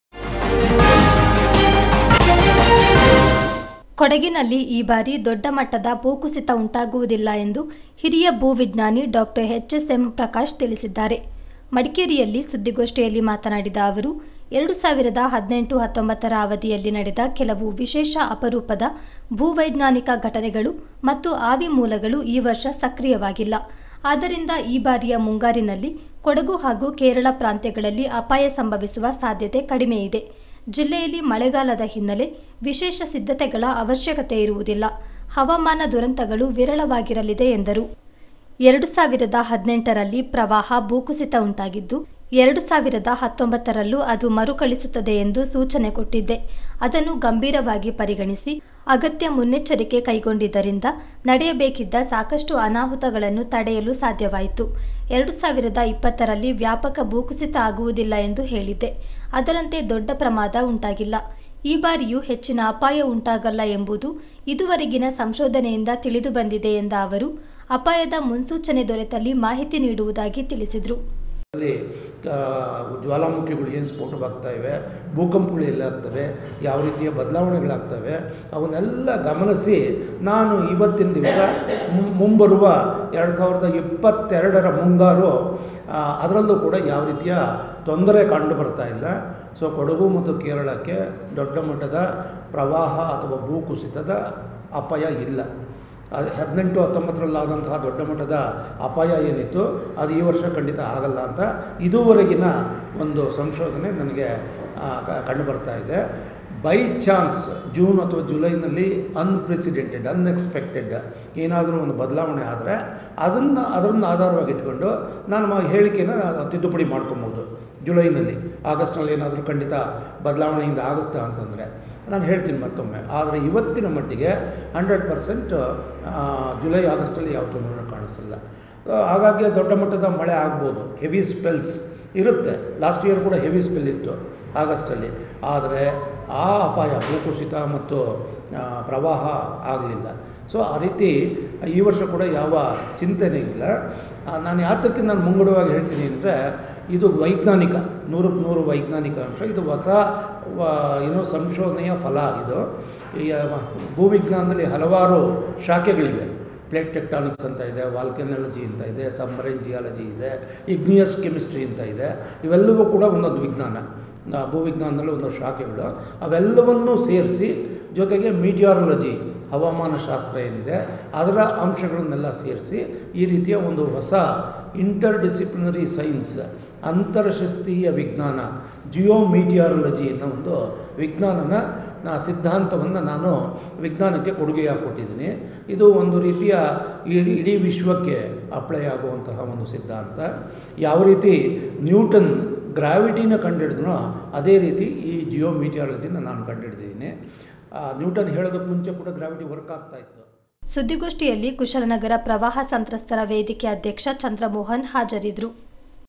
In Kodagu this time there is no major landslide ಕೊಡಗಿನಲ್ಲಿ ಈ ಬಾರಿ ದೊಡ ಮಟ್ಟದ ಭೂಕುಸಿತ ಉಂಟಾಗಲ್ಲ-
; Senior Geologist Dr. HSM Prakash ಚಿತ್ತಾರ ತಾಜಾ ಸುದ್ದಿ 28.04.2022| ಹಿರಿಯ ಭೂವಿಜ್ಞಾನಿ ಡಾ.ಹೆಚ್.ಎಸ್.ಎಂ ಪ್ರಕಾಶ್